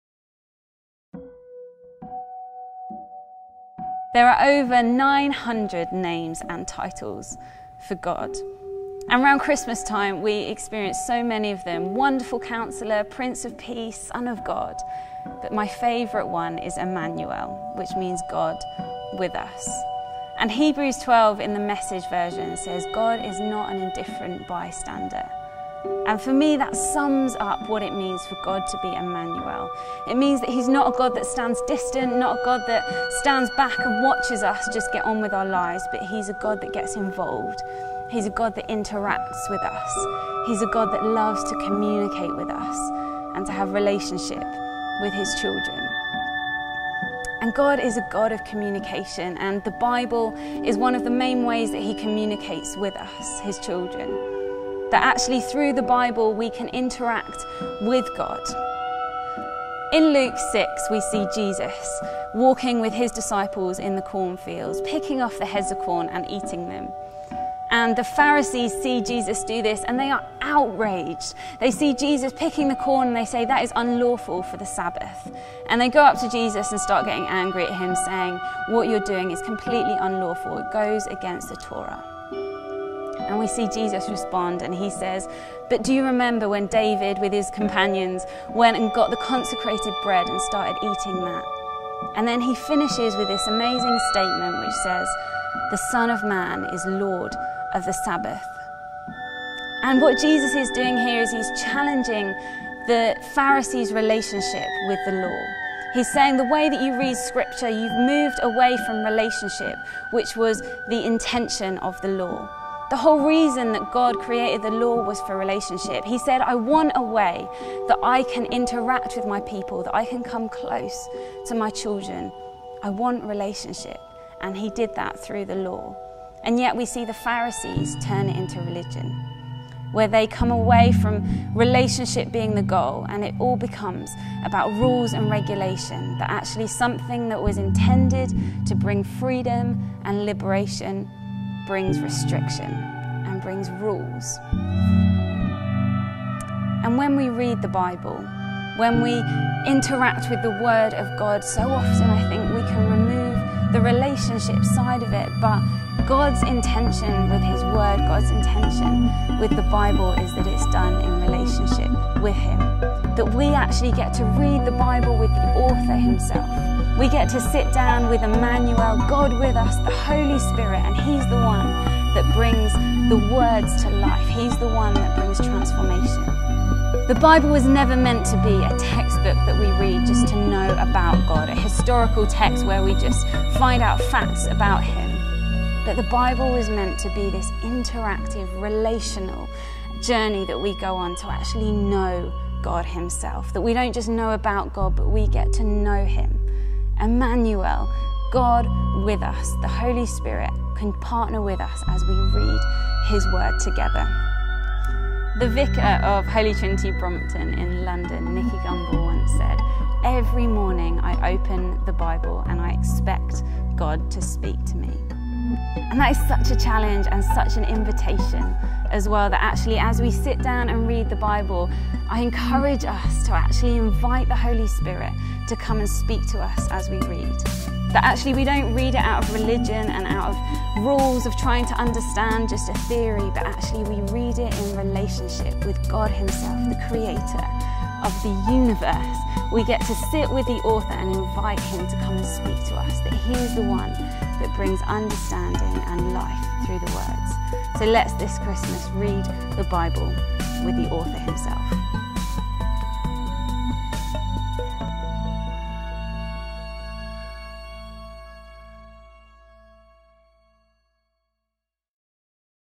The Word - Episode Four: Reading the Word // Advent Podcasts 2015
Read the Bible with the author himself. Sarah Boswell explores reading the Bible actively with the Holy Spirit